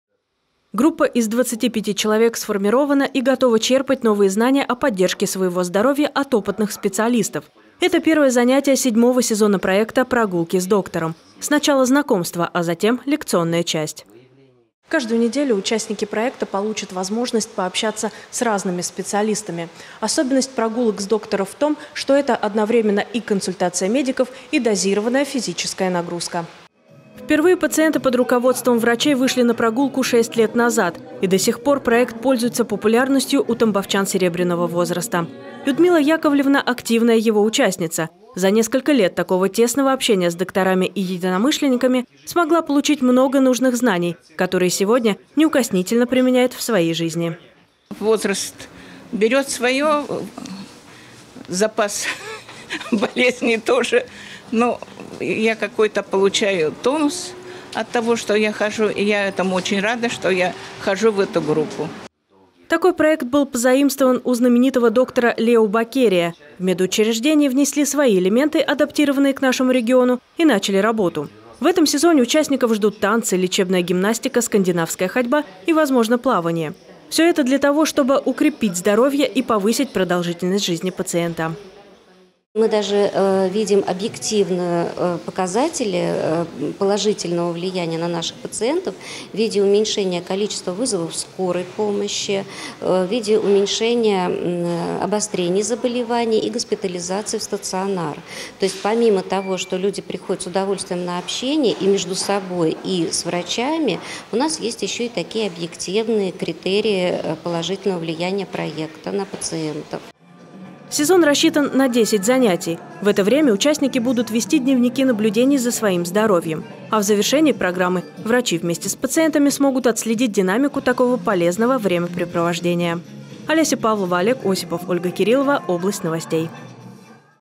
В Тамбове стартовал новый сезон проекта «Прогулки с доктором»
Он включает в себя 10 еженедельных встреч и объединяет тамбовчан пожилого возраста. В это время участники будут вести дневники наблюдений за своим здоровьем. А в завершении программы врачи вместе с пациентами смогут отследить динамику такого полезного времяпрепровождения.